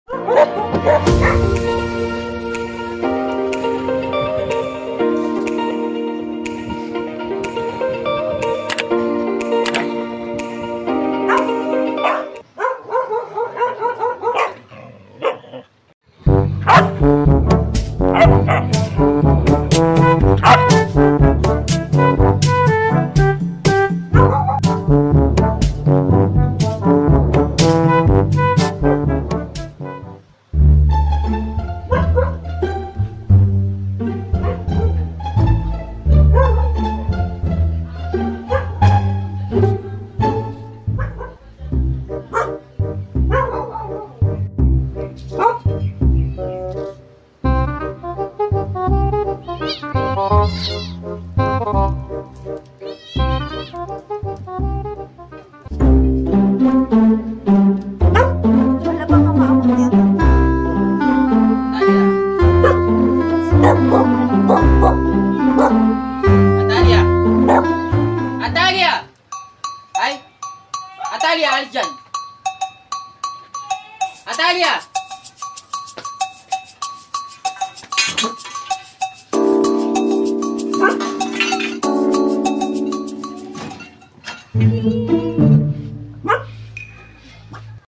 Inampong Mga Tuta Nagsama Pa Ng Kaibigan Nilang Pusa😁🤣
Dahil Iniwan Ng Nanay inampon Nalang Ang Mga Tuta At  Sinama Na Rin Pati Kaibigan Nilang Pusa